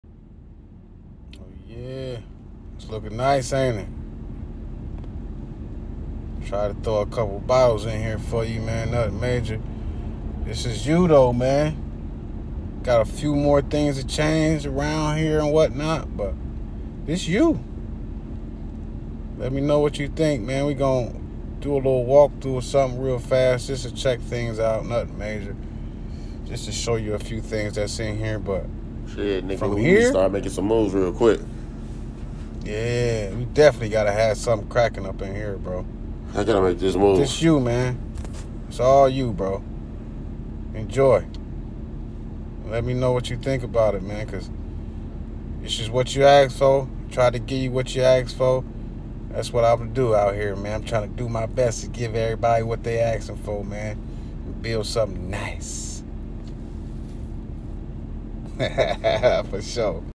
Time to make moves
Run through the digital metaverse with Chitraka and Friends as they explore web3 from an animated point of view. These images reflect in real life scenes that are free-styled using multiple applications to make things feel natural but digital at the same time. All characters have been given the authentic space to express themselves based on their own personalities.

These scene are created to have fun in the digital space to create a new experience for anyone interested in expanding their brands or content.

Building structures used in these scenes are either downloaded from a free platform as assets or self molded in the Blender application.

In this scene Chitraka has brings Izzy to the new club for the city The Losers Lounge.